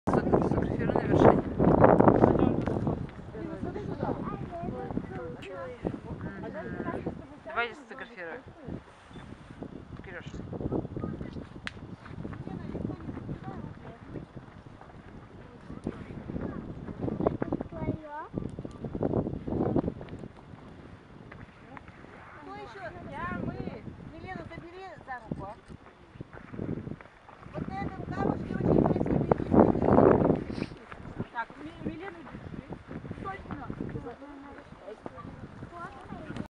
Вид с Тренькинской скалы
Вид с Тренькинской скалы. Северный Урал. Пыталась снять панораму Денежкина камня на горизонте, но из-за пролетающего снега не получилось. Под скалой старое высохшее русло реки Сосьва.